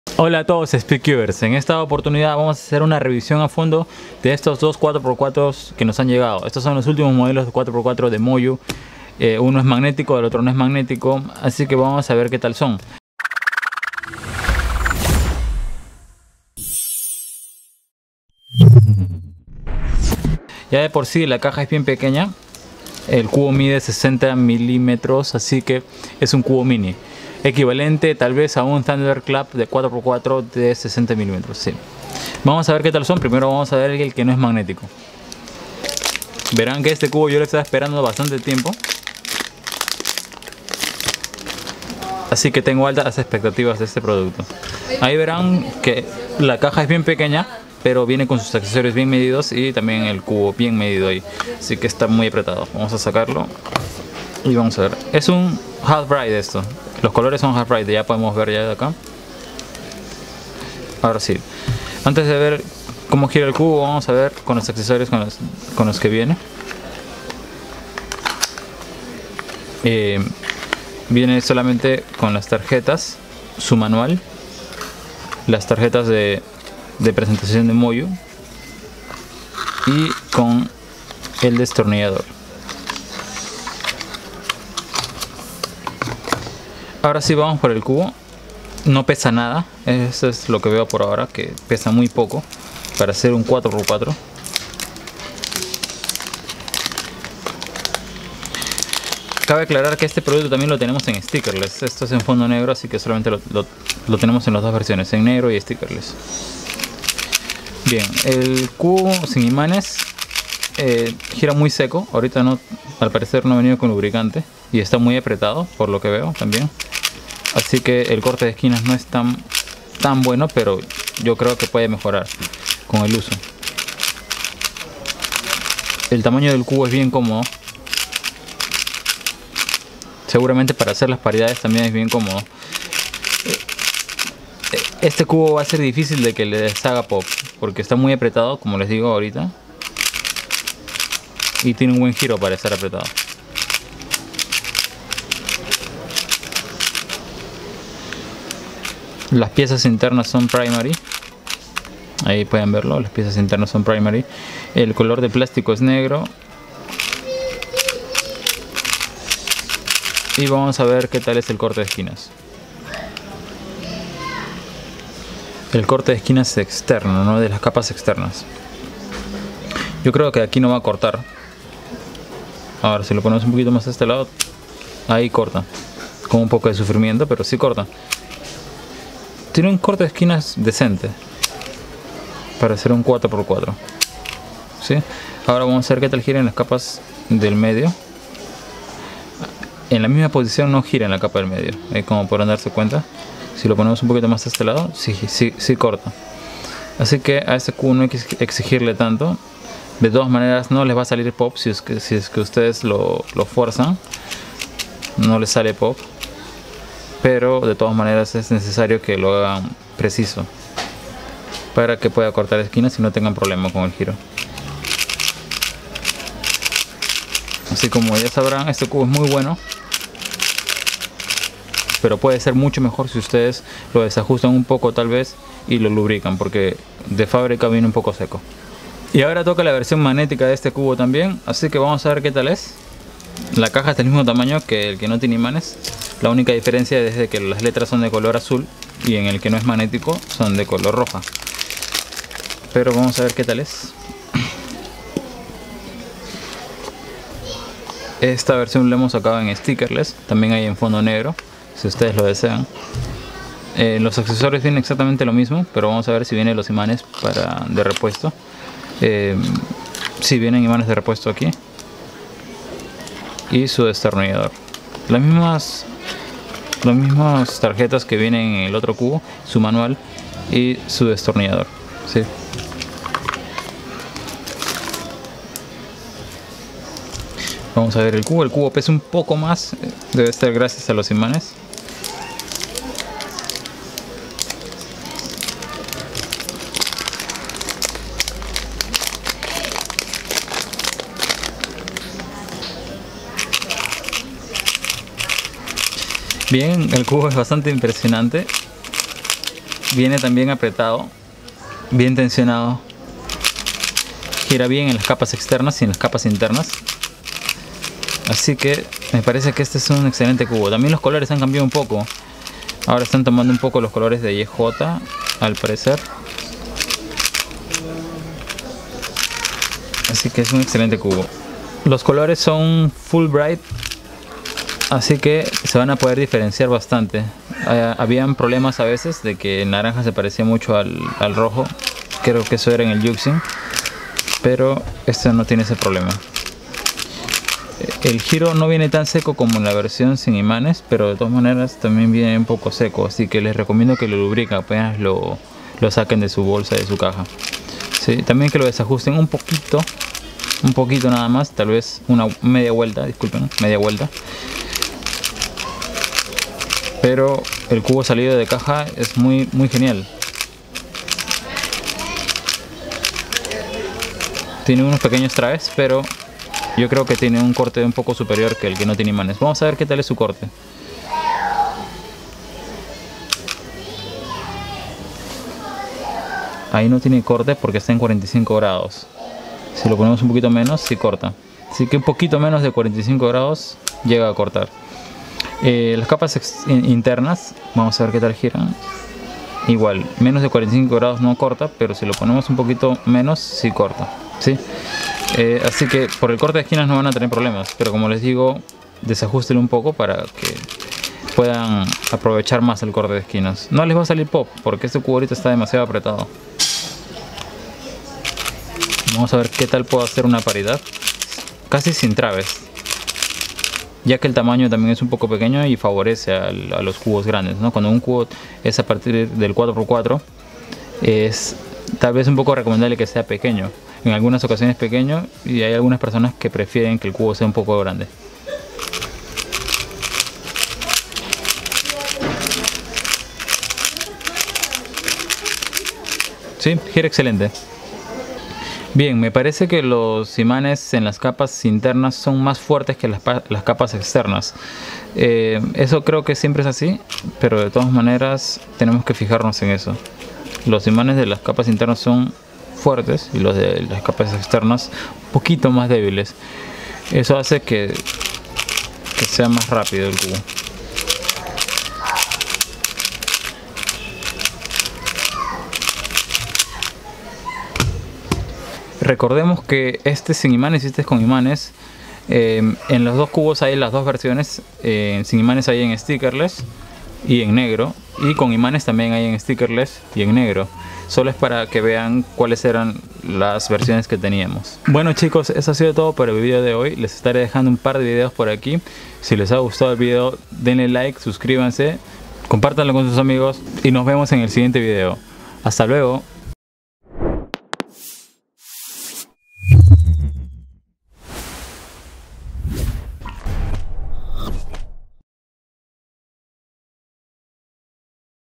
MoYu Aosu WR & WR Magnético Review en ESPAÑOL | Speedcuber Perú
Tu tienda favorita de cubos te trae una review en español de los nuevos 4x4 MoYu Aosu WR los cuales son unos excelentes cubos para hacer speedcubing y en el video te mostramos a detalle sus especificaciones